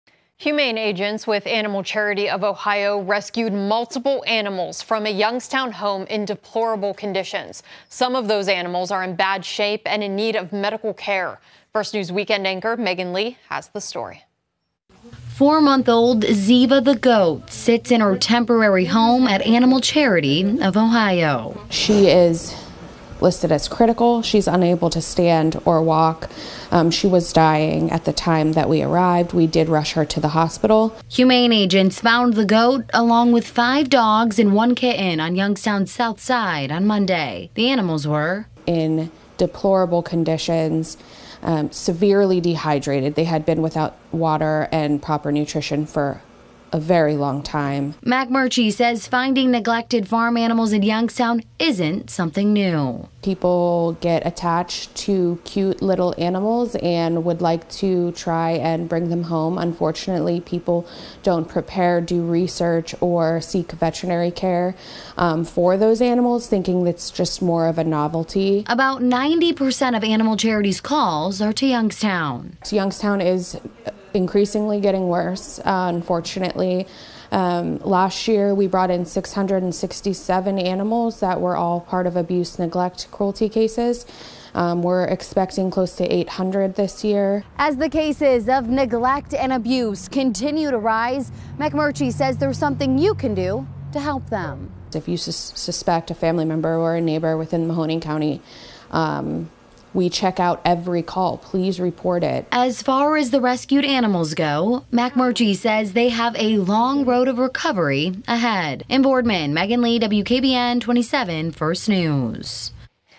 Multiple animals removed from Youngstown home, including goat
Multiple animals removed from home on Youngstown, Ohio's South Side, including goat